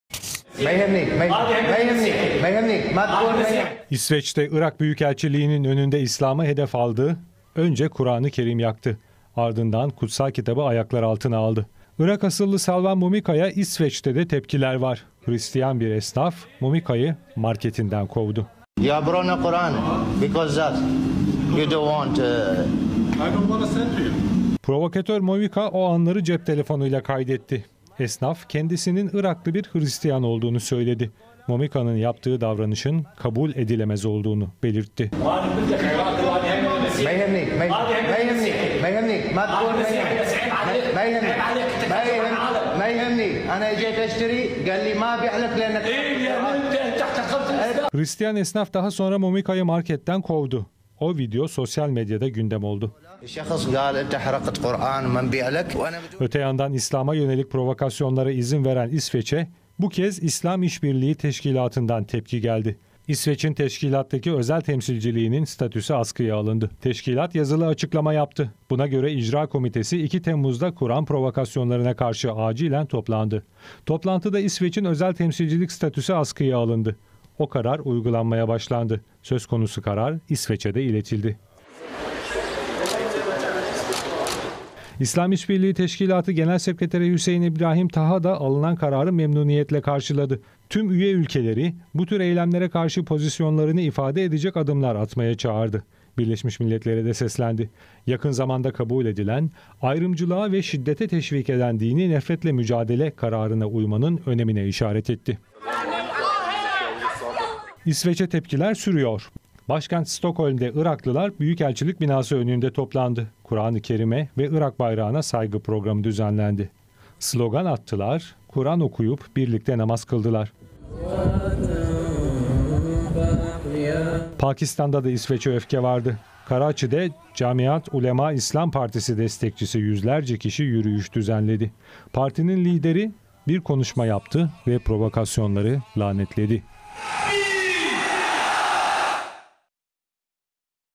Kuran-ı Kerim Yakmıştı! Hristiyan Esnaf O Küstahı Dükkandan Kovdu!
İsveç'teki Irak Büyükelçiliği’nin önünde İslam’ı hedef aldı. Önce Kur'an-ı Kerim'i yaktı. Ardından kutsal kitabı ayaklar altına aldı. Irak asıllı Salvan Momika'ya İsveç'te de tepkiler var. Hristiyan bir esnaf Momika'yı marketinden kovdu.